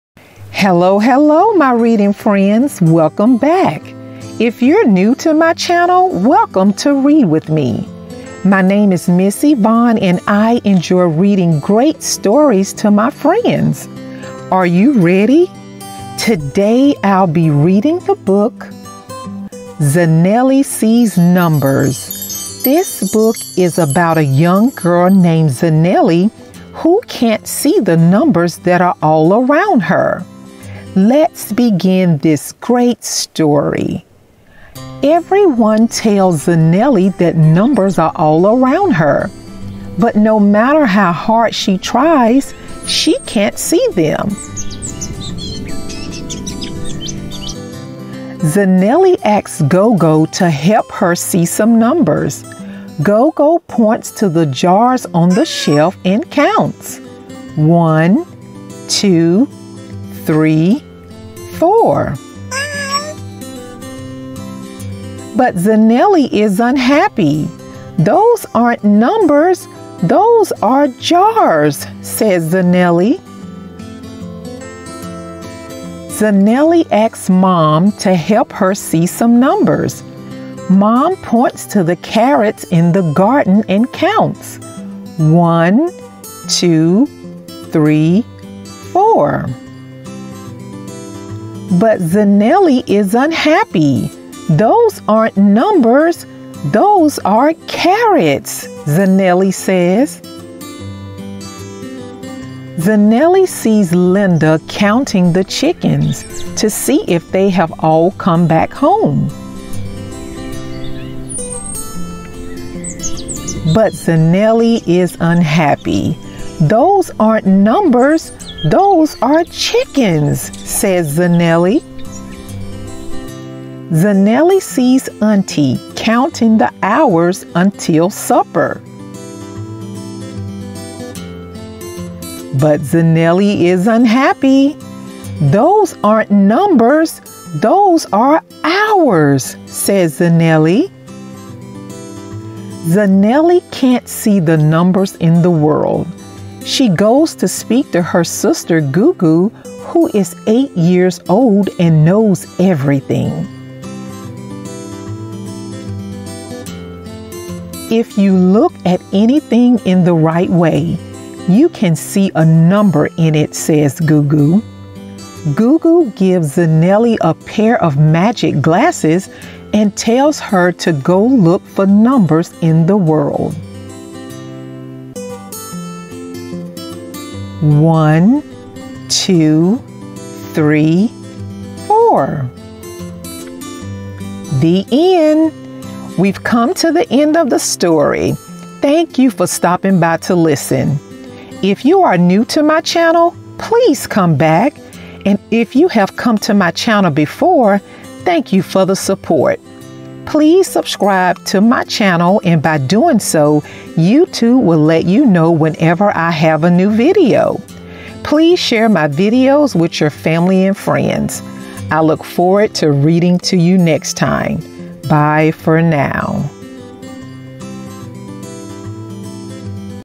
Zanele Sees Numbers (Short Story For Kids) Short Story In English Read Aloud |Story Time |Animation
In this short story for kids in English: Read aloud | Children's book | Story time | Short story animation | Kid book stories | Bedtime story | Short story for kindergarten

"There are numbers everywhere, but Zanele can't see them."

Children's book:
Written by: Laura de Lange
illustrated by: Hanrie Coetzee
Edited by: Catherine Parkinson
Designed by: Arthur Attwell

Changes have been made to the original work.

Musician: @iksonmusic

No changes have been made to the music